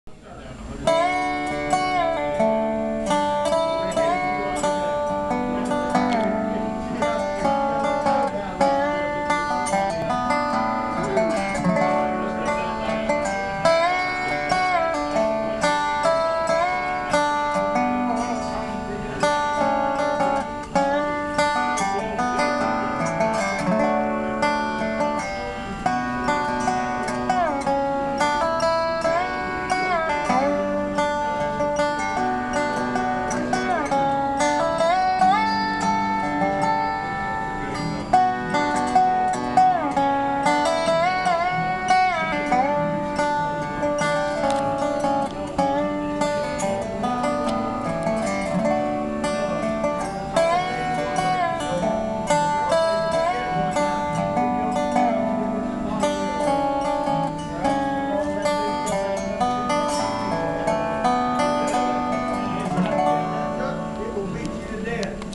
Chris on the Dobro
Chris doing a tune on the Dobro. At the Barn in Baldwin Florida